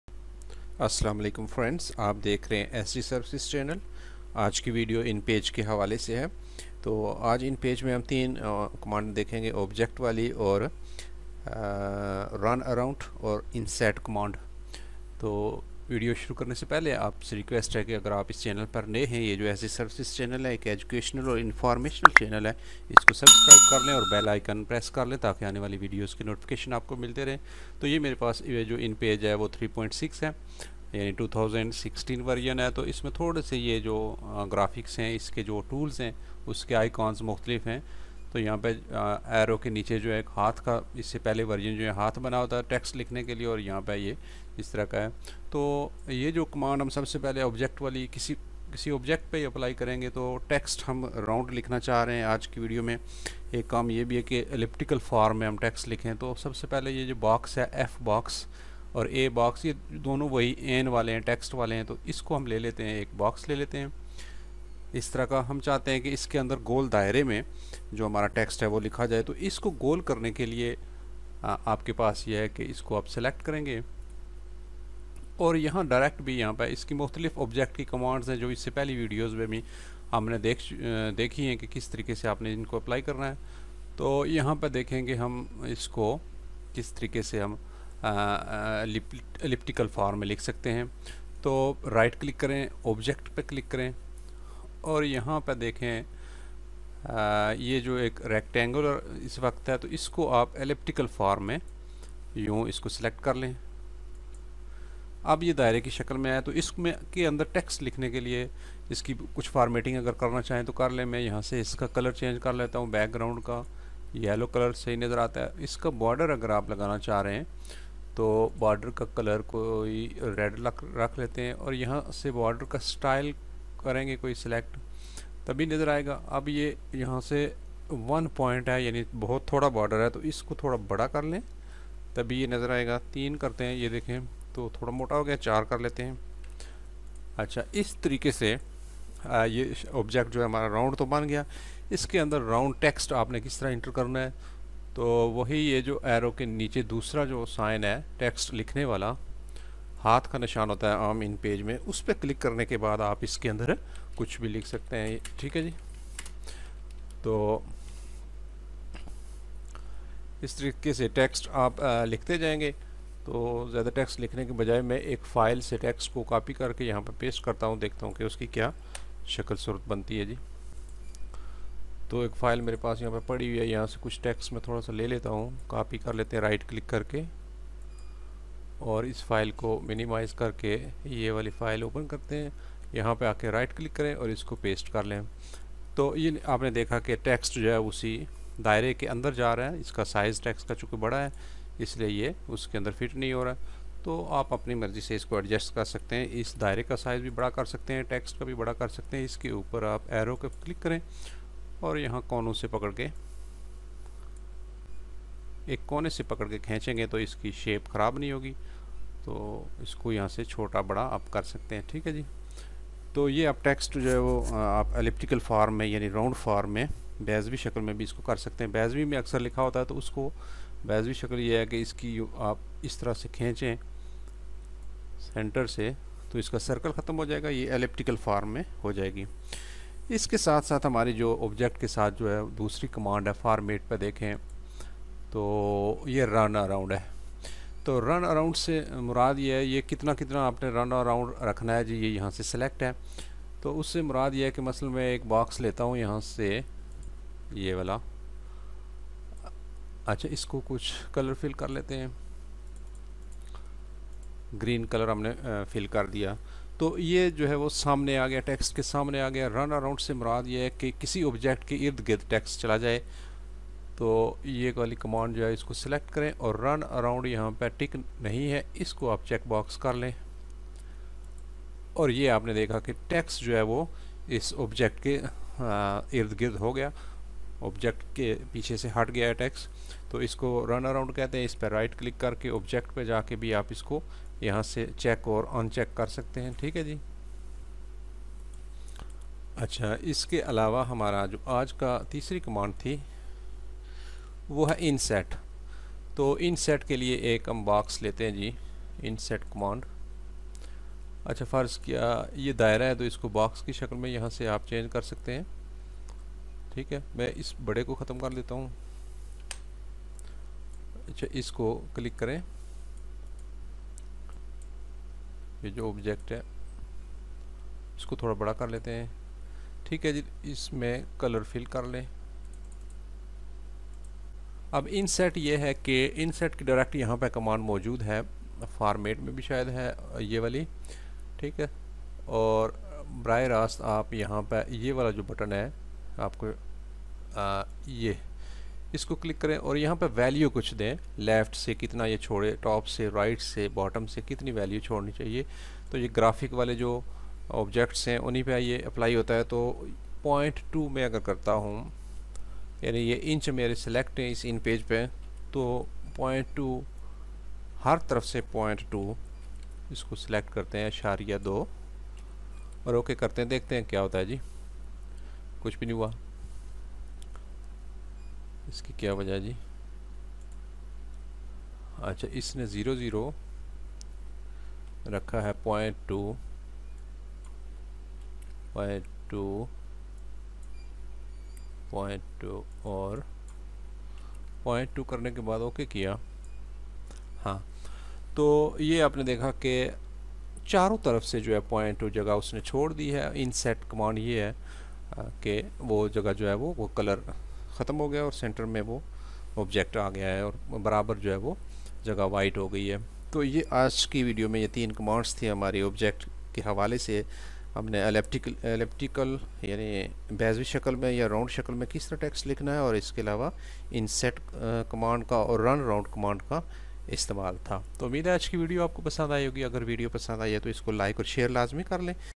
How to write in circle in Inpage | Text in circle in Inpage | Use of Object,Runaround,Inset Commands
In this video the method of writing text in circle box or elliptical box is told. Object, Runaround and Inset commands are explained in detail.

1) This video has no negative impact on the original works (it would actually be positive for them)
2)This video is also for Teaching purposes.